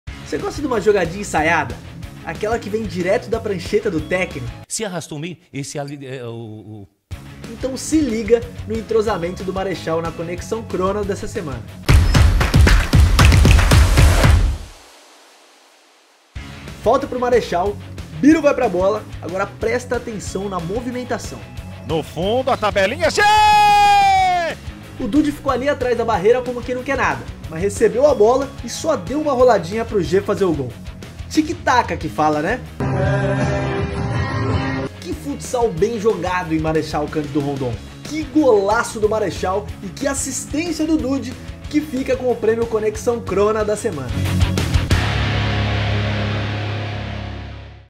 LNF2021 - Conexão Krona - Semana 04
Dudi, do Marechal, deu a assistência mais bonita da 4ª Semana da LNF 2021